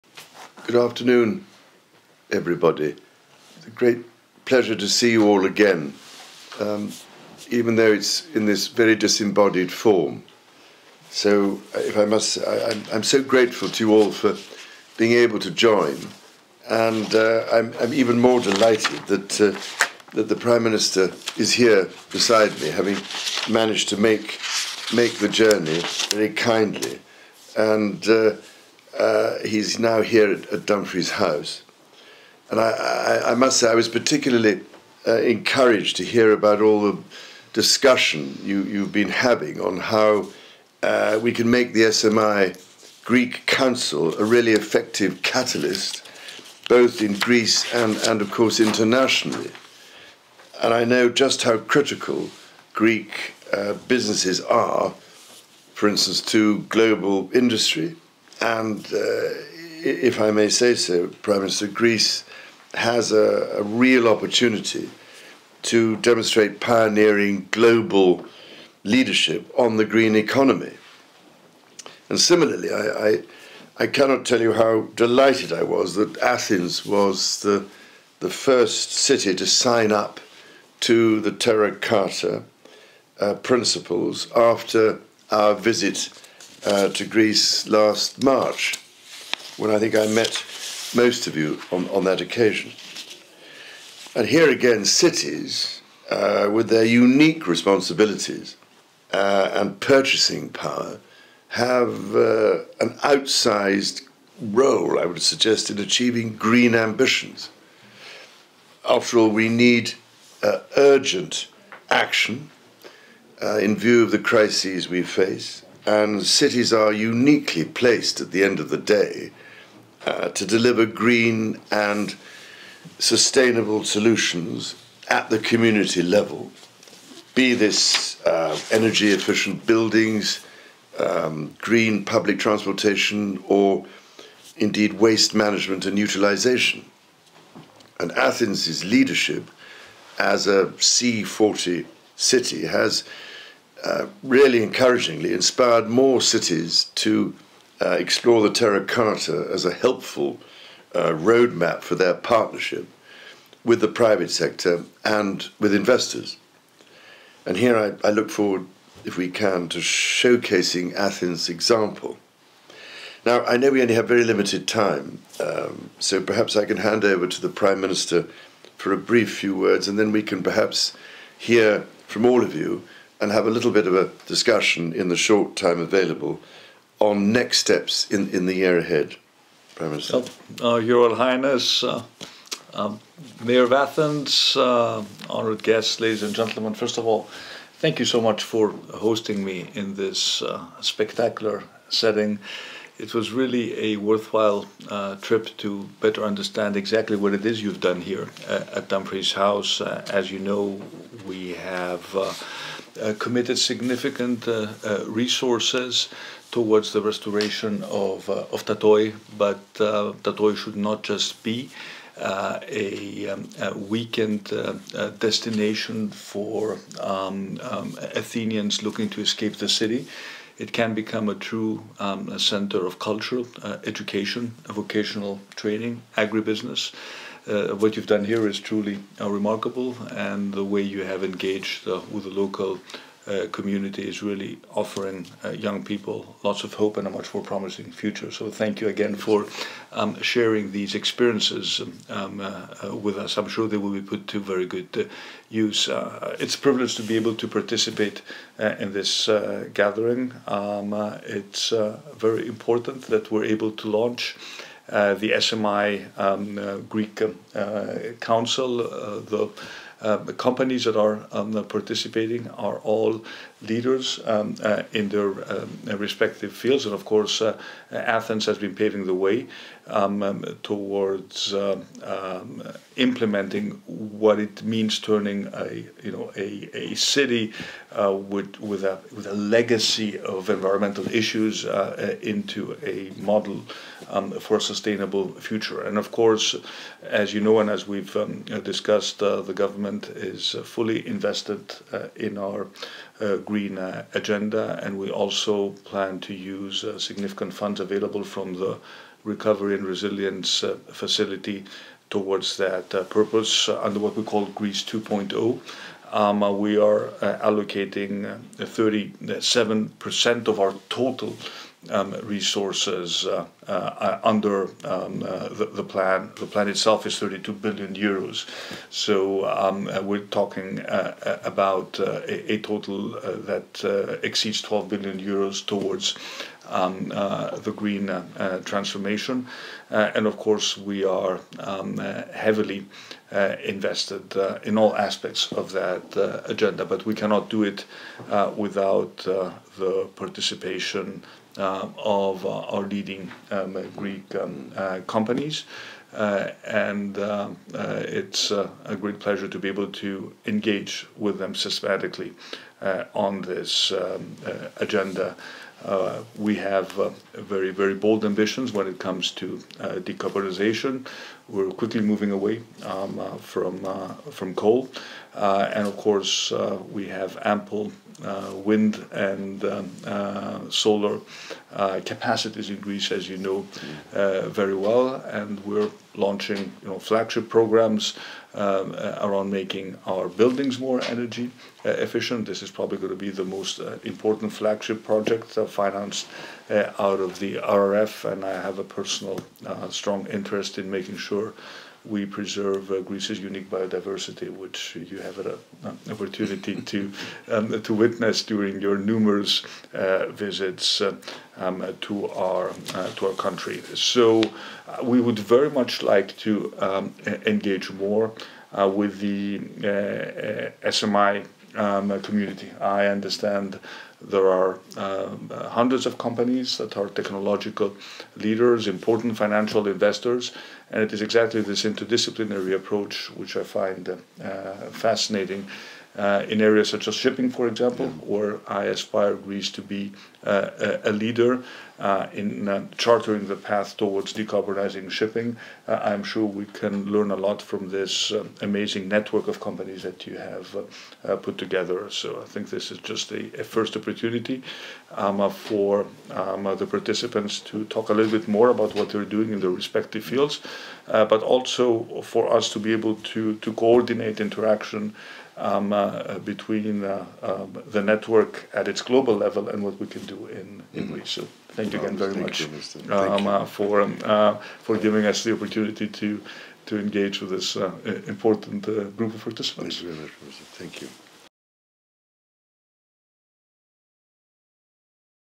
Συμμετοχή του Πρωθυπουργού Κυριάκου Μητσοτάκη στην τηλεδιάσκεψη με το Ελληνικό Συμβούλιο SMI
Ο Πρωθυπουργός Κυριάκος Μητσοτάκης και ο Πρίγκιπας της Ουαλίας Κάρολος έλαβαν σήμερα μέρος σε τηλεδιάσκεψη με το Ελληνικό Συμβούλιο του οργανισμού Sustainable Markets Initiative (SMI) του Πρίγκιπα, ο οποίος αποβλέπει στην αναγνώριση των θεμελιωδών δικαιωμάτων της φύσης και στην οικοδόμηση ενός βιώσιμου, αειφόρου μέλλοντος.